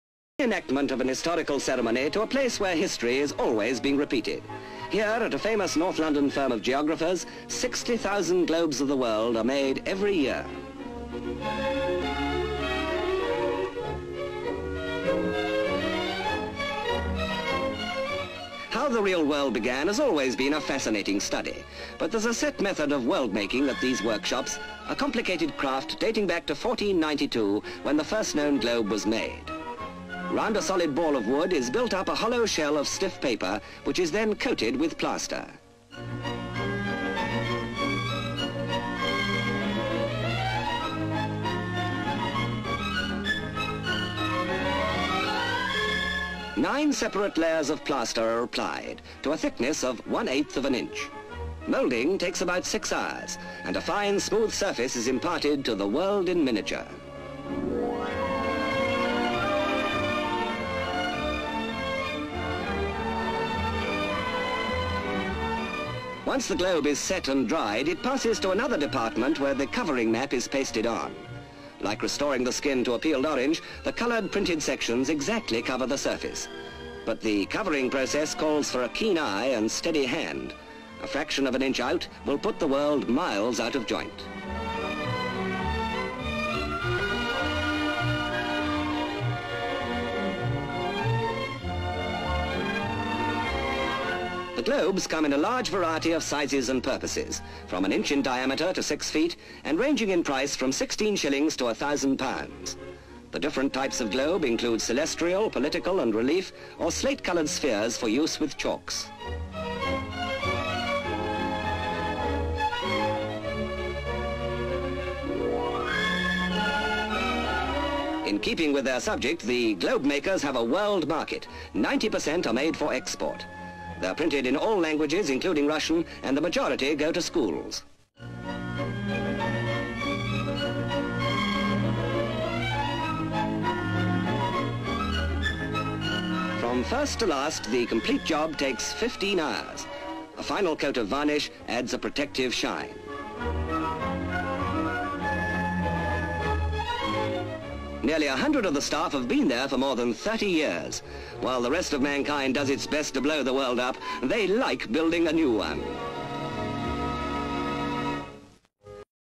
Globes 1955
This Pathe newsreel shows how world globes were made in 1955.